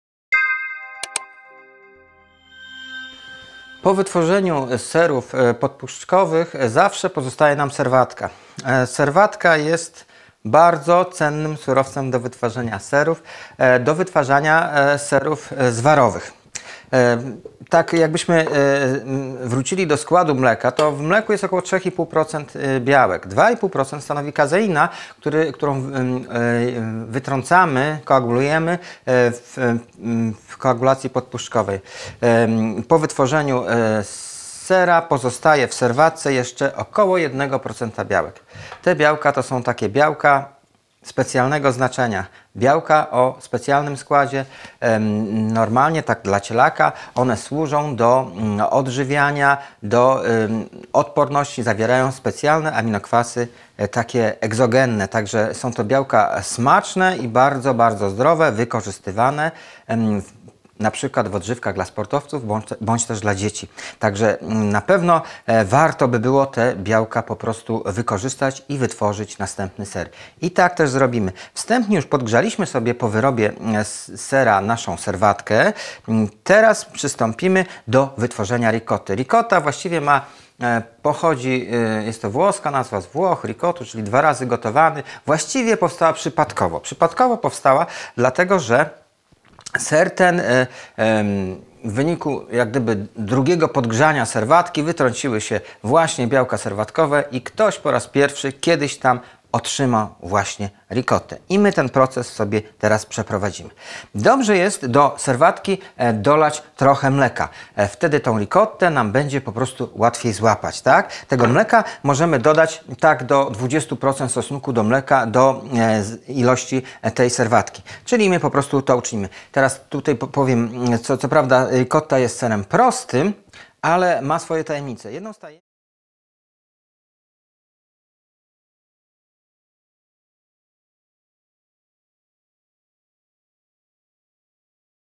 Serowarstwo domowe cz. 1. - "Sery zwarowe" - fragment szkolenia
Sery zwarowe są bardzo ciekawą kategorią serów. Można powiedzieć, że są niejako produktem ubocznym powstawania innych serów. Powstają bowiem z serwatki, pozostającej po wyrobie serów podpuszczkowych np. Goudy, Fety, Oscypka, czy Mozarelli.

Serowar Żuławski - Krzysztof Jaworski na filmie szkoleniowym pokazuje, w jaki sposób wykonać najpopularniejsze ser zwarowy "Ricottę". Ser ten świetny jest na świeżo na śniadanie lub jako dodatek do wypieków, czy klusek. Prócz znanej Ricotty, na filmie zobaczysz również wytwarzanie włoskiego Mascarpone - sera o coraz większej popularność. Popularność tę zdobywa ze względu na swoją aksamitną konsystencję i słodkawy smak. To wszystko jednak za sprawą specyficznego surowca i dodatków, z których jest wytwarzany.